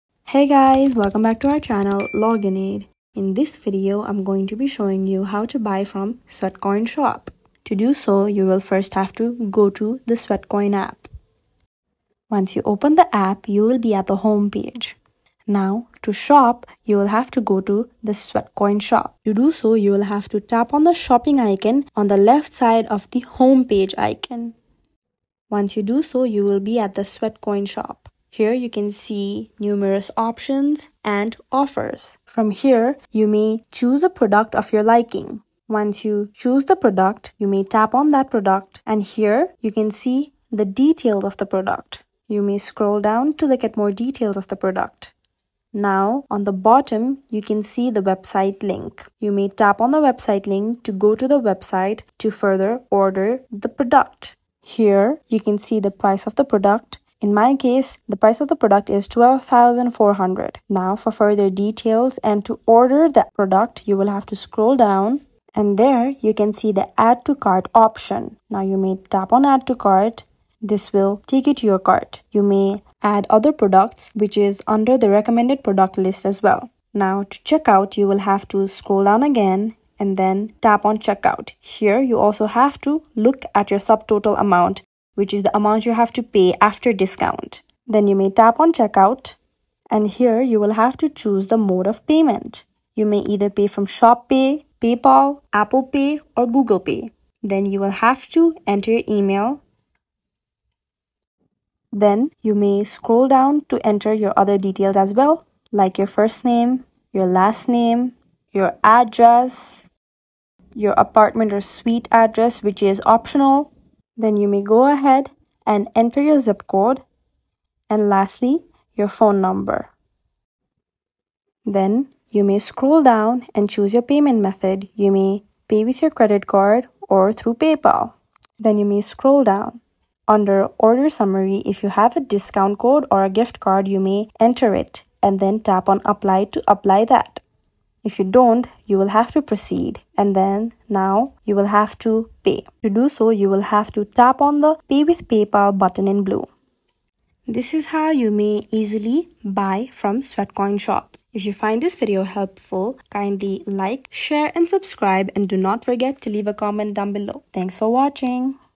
How To Buy From Sweatcoin Shop 2024 | Purchase Products On Sweatcoin App Tutorial
Discover how to buy from the Sweatcoin Shop with this easy-to-follow tutorial. It allows you to earn rewards for your steps and purchase products from their shop. Learn how to navigate the Shop, select items, and complete your purchase. Start shopping with your Sweatcoins today!